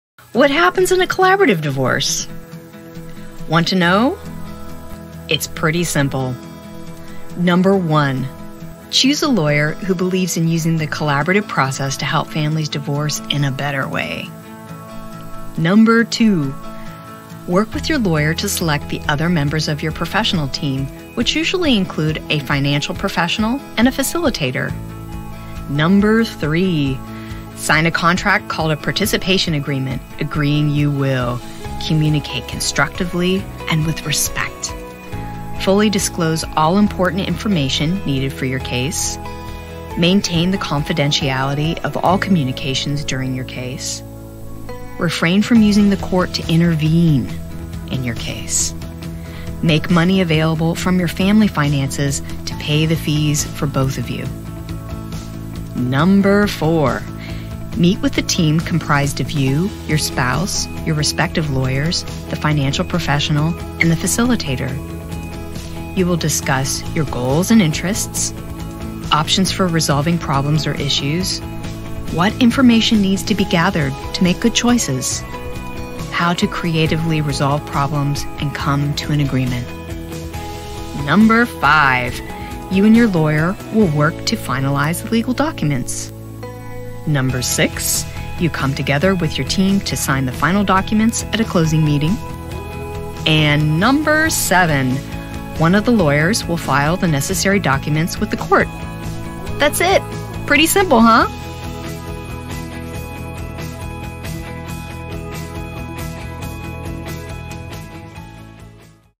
Leap Frog Divorce: Want to Know What Happens in a Collaborative Divorce
What happens in a collaborative divorce? It's really simple.

First, choose a lawyer that believes that the collaborative process helps families to divorce in a better way. Then use your lawyer to help you choose other members of your professional team.

Once your team is in place, you will sign a participation contract which basically says you will cooperate and collaborate with your spouse through the divorce proceedings. You will communicate with respect, disclose all important information, maintain confidentiality, refrain from using the court, use family finances to pay fees for both of you.

Once that has been signed you will meet with your team and discuss the agenda for your divorce. The important thing to remember is that this is all dictated by you and your spouse. Your rules, your schedule, etc.

When everything has been settled, you and your team will finalize the agreements and you will sign them. After that a lawyer will send the necessary documents to the court and you're done!